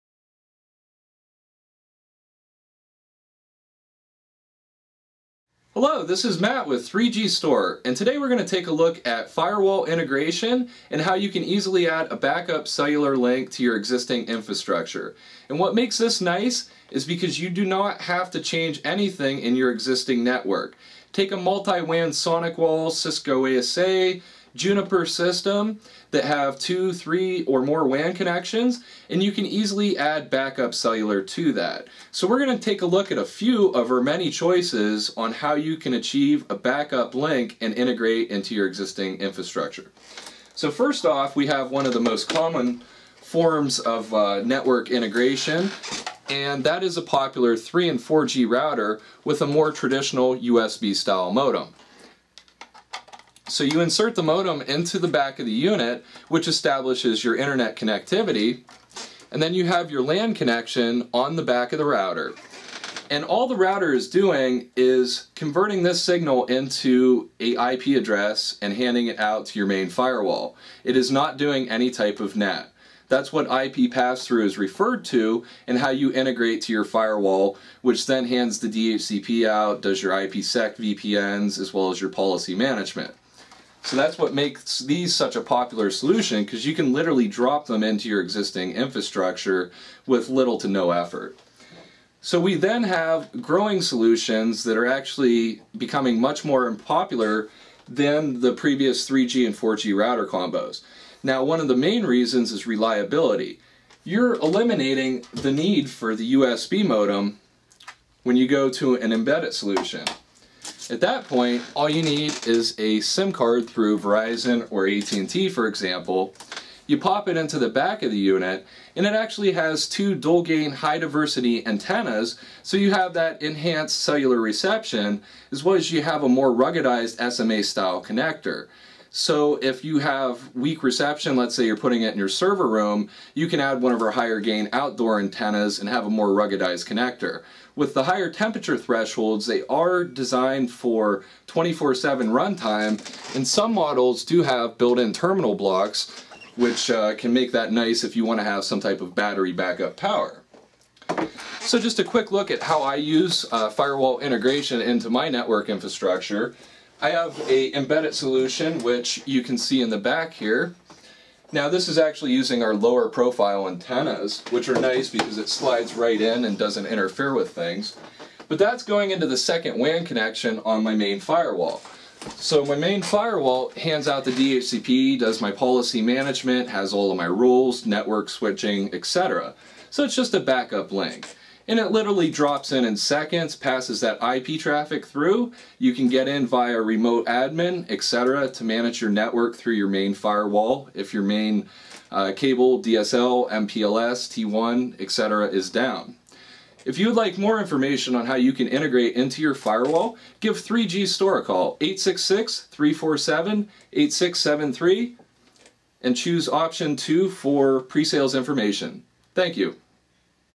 3G & 4G Router Firewall Integration - 3Gstore.com
Are you an enterprise customer looking for a router that can integrate with your existing firewall?  Let 3Gstore show you how.